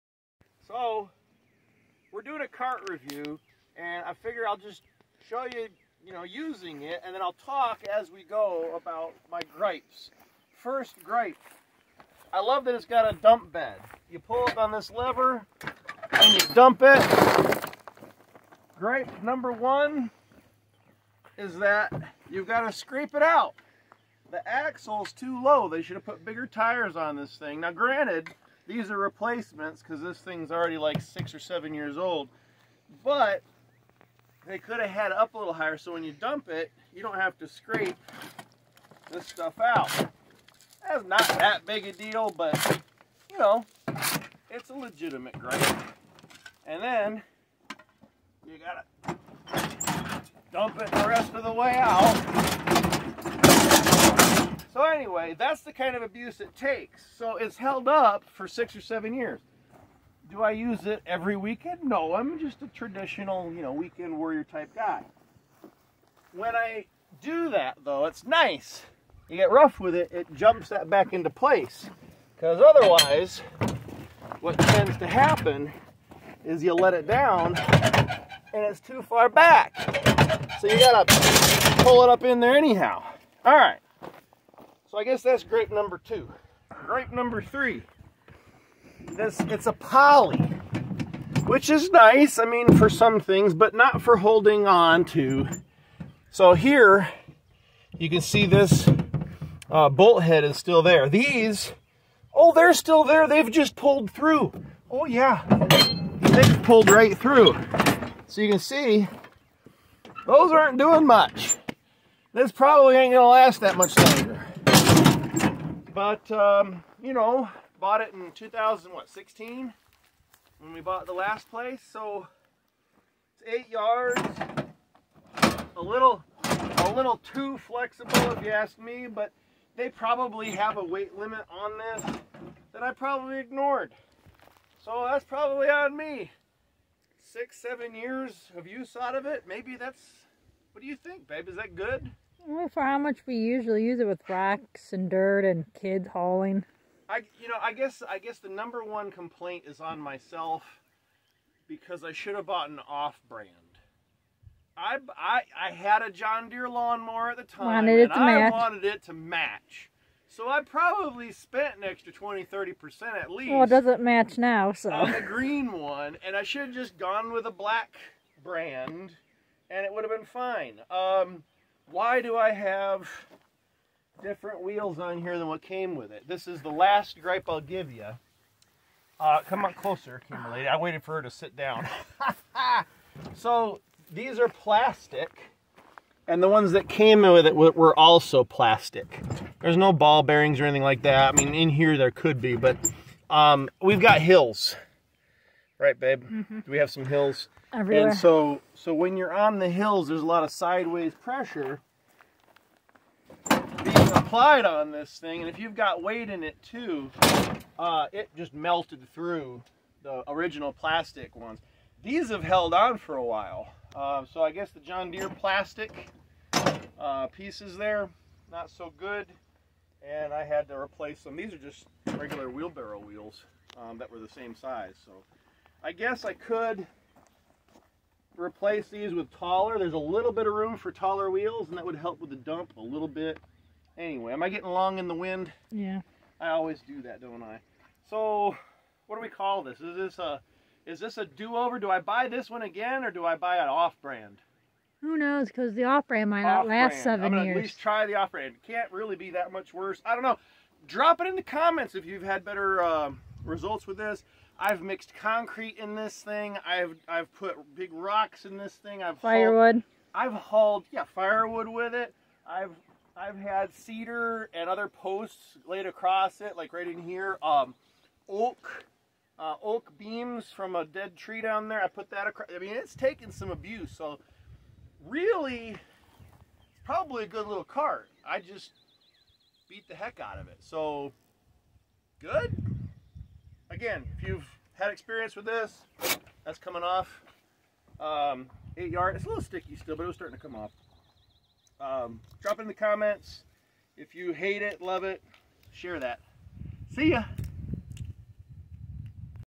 John Deere poly yard cart, 6 year review: To be fair, it's taken some abuse!
Oh yeah...it converts to a push cart, too.  I rarely used it that way, though...the stand would get in the way...best to leave it in tow mode (in my experience).